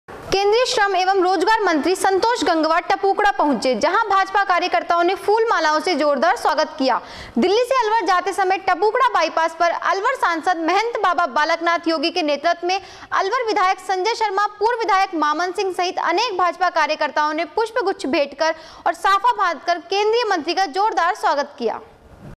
alwar news//केंद्रीय मंत्री गंगवार का टपूकड़ा में किया स्वागत//20.5.2021
केंद्रीय श्रम एवं रोजगार मंत्री संतोष गंगवार का टपूकड़ा पहुँचे।जहा  भाजपा कार्यकर्ताओं ने फूल मालाओं से जोरदार स्वागत किया। दिल्ली से अलवर जाते समय टपूकड़ा बाईपास पर अलवर सांसद महंत बाबा बालक नाथ योगी के नेतृत्व में अलवर विधायक संजय शर्मा, पूर्व विधायक मामन सिंह सहित अनेक भाजपा कार्यकर्ताओं ने पुष्प गुच्छ भेंट कर और साफा बांधकर केंद्रीय मंत्री का जोरदार स्वागत किया।